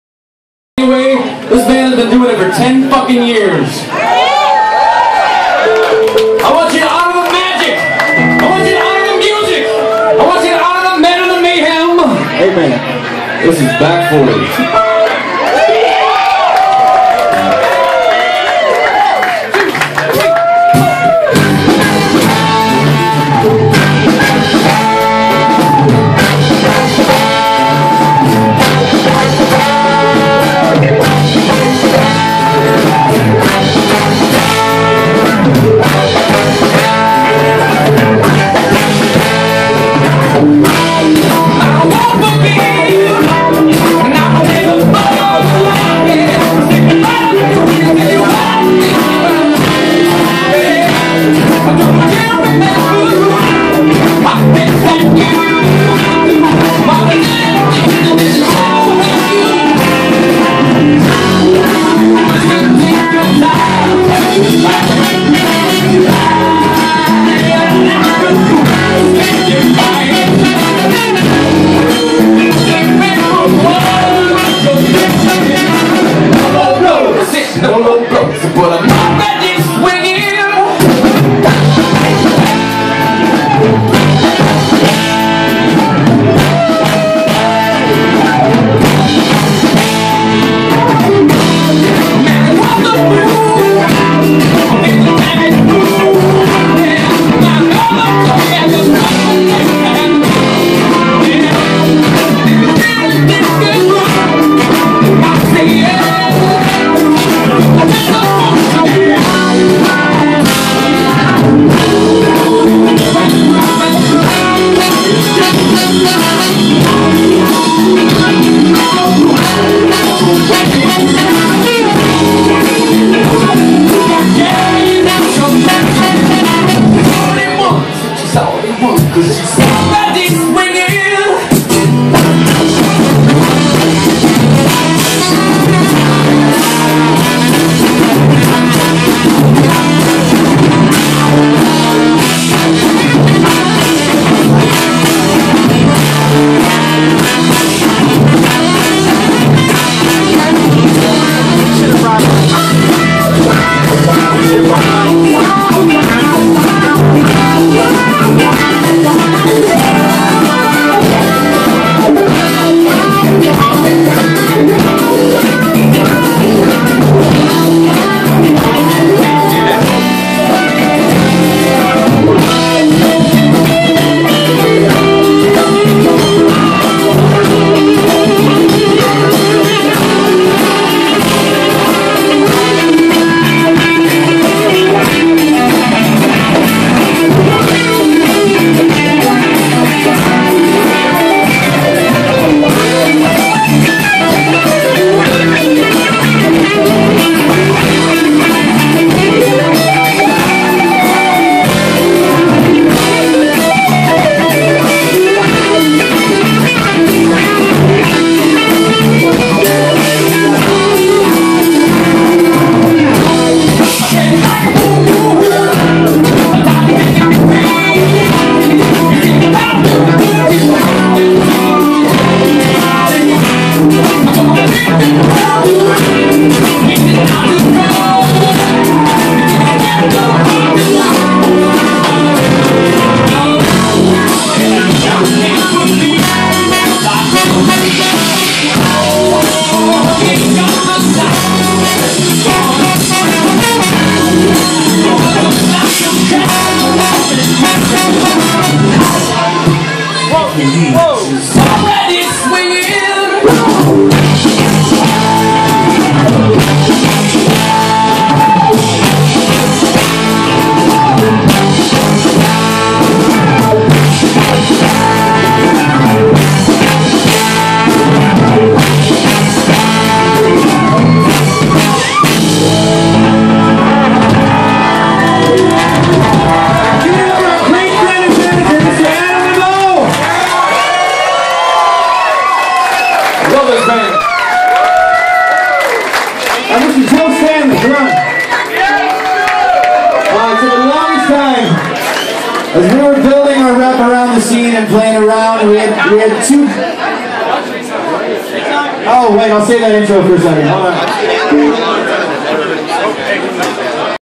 20130331_000838 Adam Labeaux sits in at Back Forty's Last Show - Blind Pig - Ann Arbor MI
What a great performance and night!  WOW!

Adam kicked ASS!

Use the Video if you want - the audio blows.